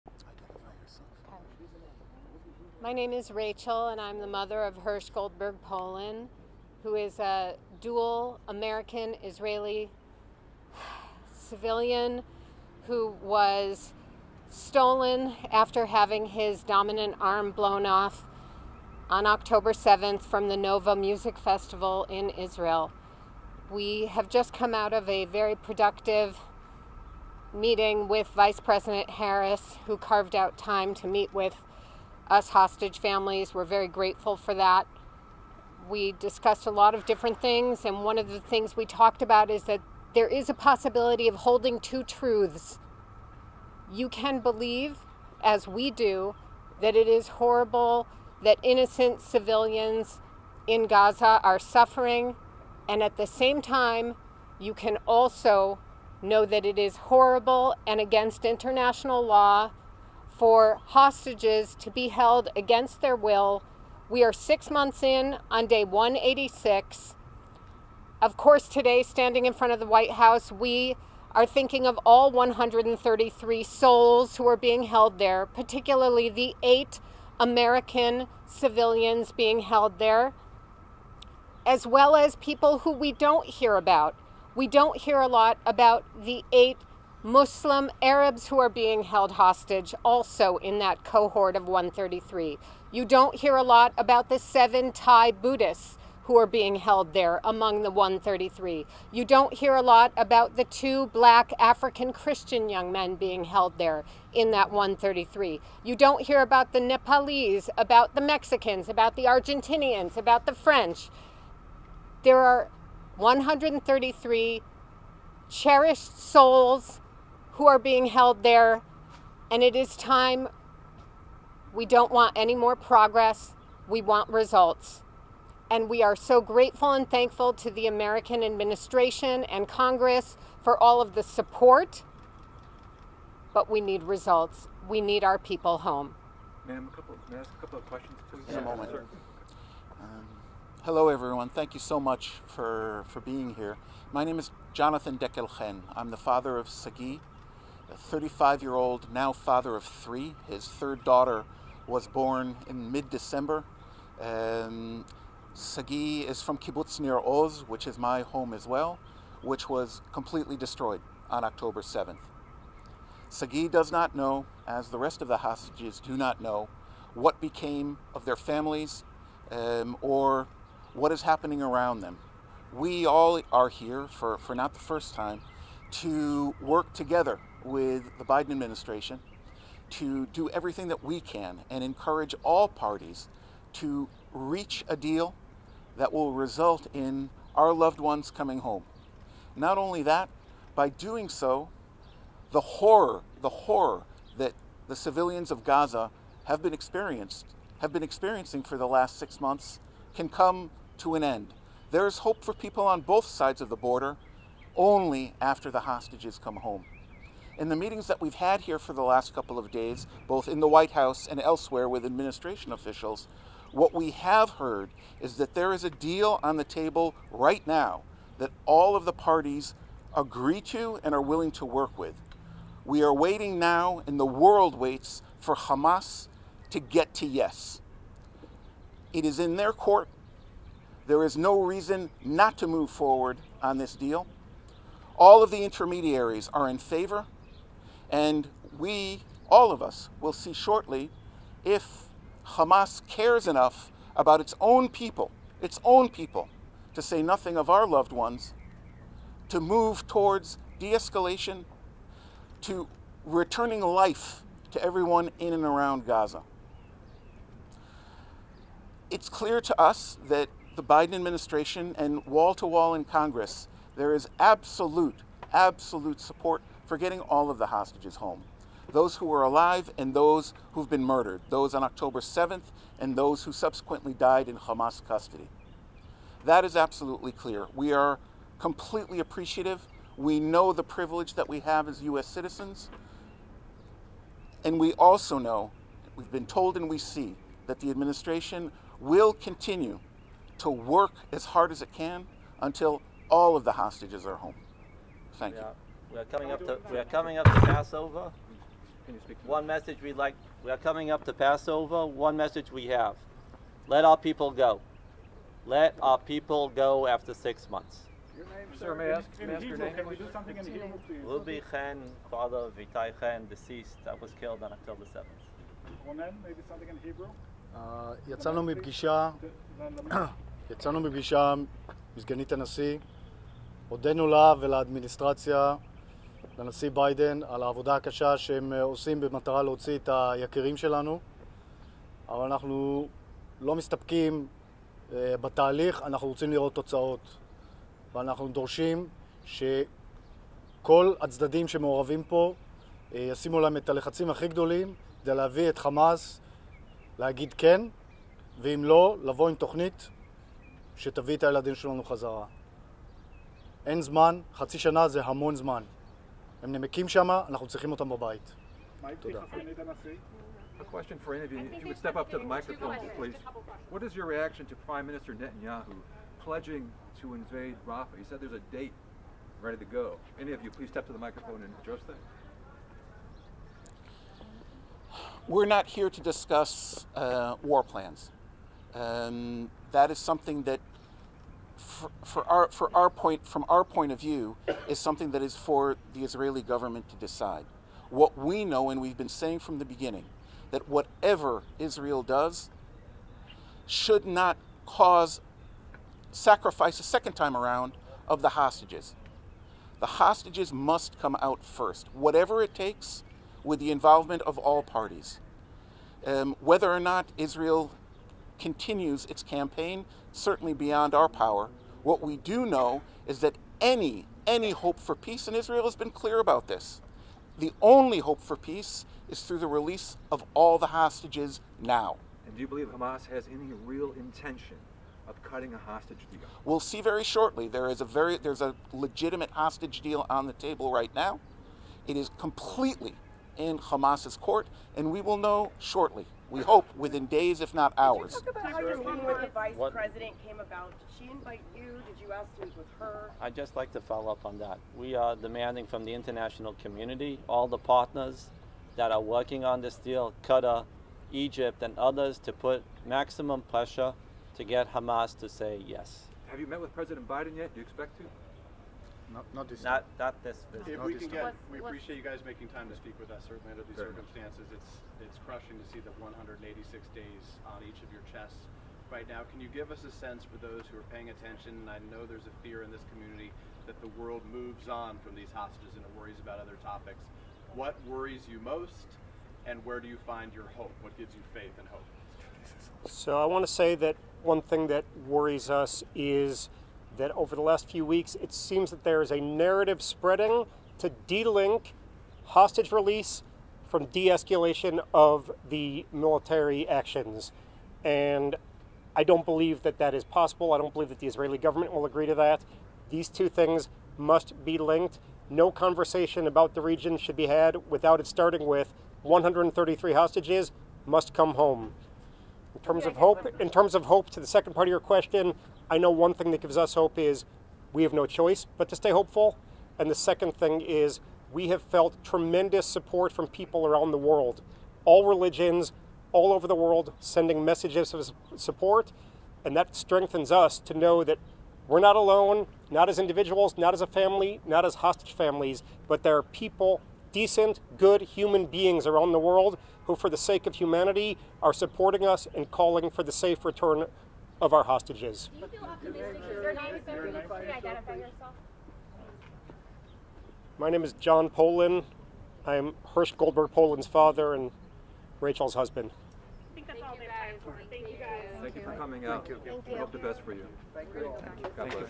Families Of U.S. Citizens Taken Hostage By Hamas Speak To Reporters After Meeting With VP Harris
The families of U.S. citizens held hostage by Hamas speak to reporters outside the White House after meeting with Vice President Kamala Harris.